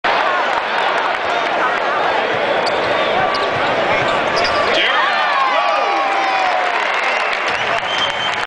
Derek Rose II
Derek Rose of the Chicago Bulls scores  v. the Philadelphia 76rs 12/2/08